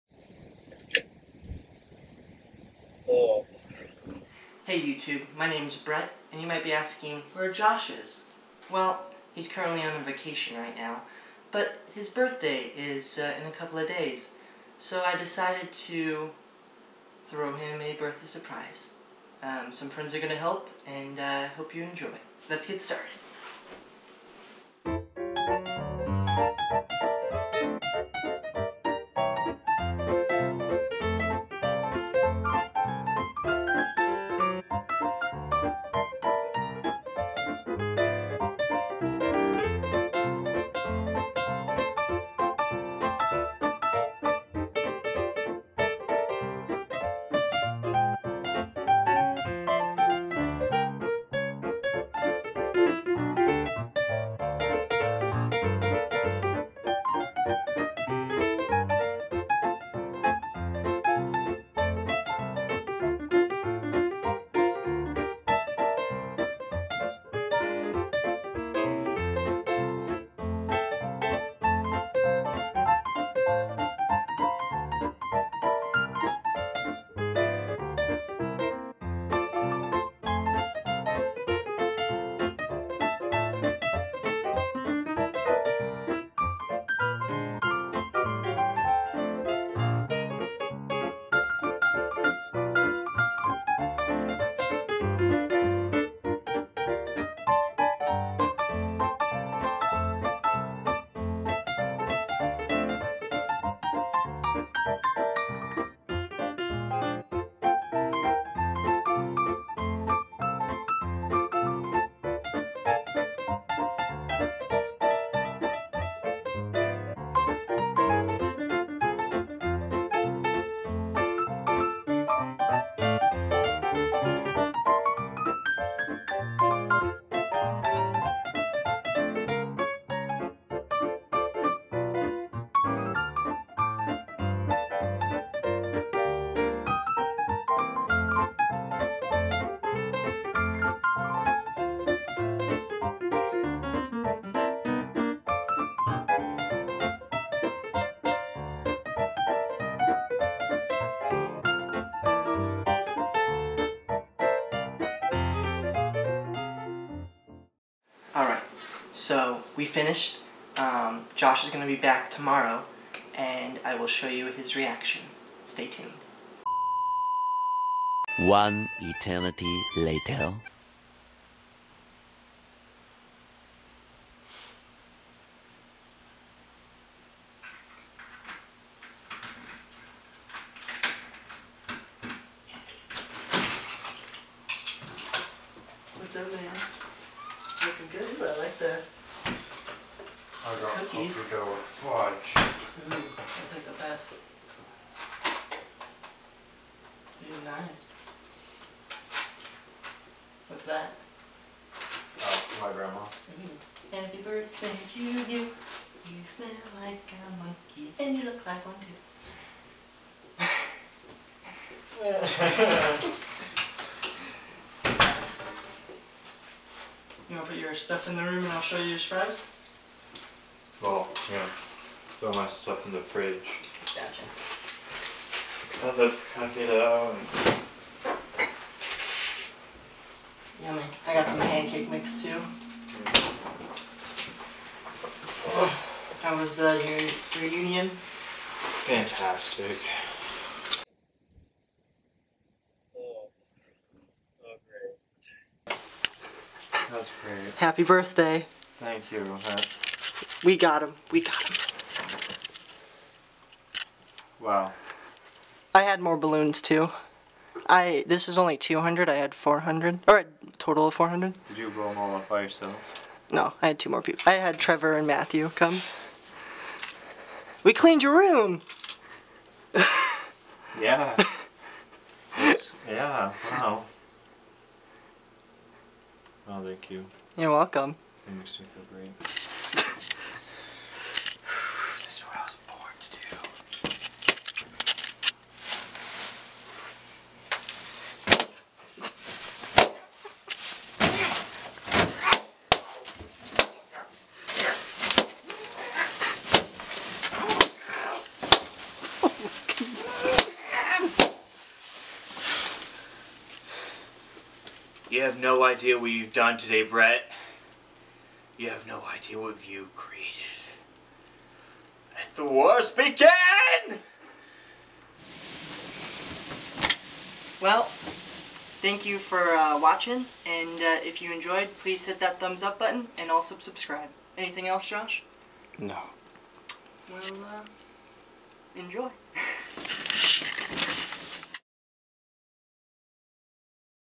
Pranking My Roommate. I Might Have Gone to Far!!! Birthday Surprise
Hey YouTube, come watch me prank Josh for his birthday! Check out our Facebook and Instagram page to keep updated with all the videos we are doing. We will be posting videos every two weeks on Saturdays.

We do not own or take credit for song, pictures, or clips.